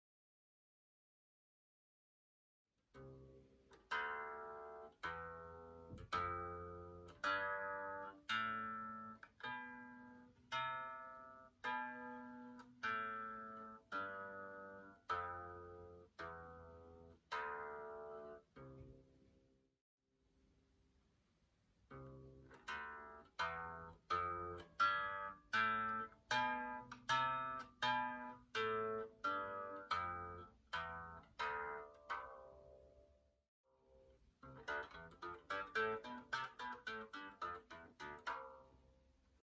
How To Play C major Scale on Bass
A quick play through of the C major scale on bass at three different speeds.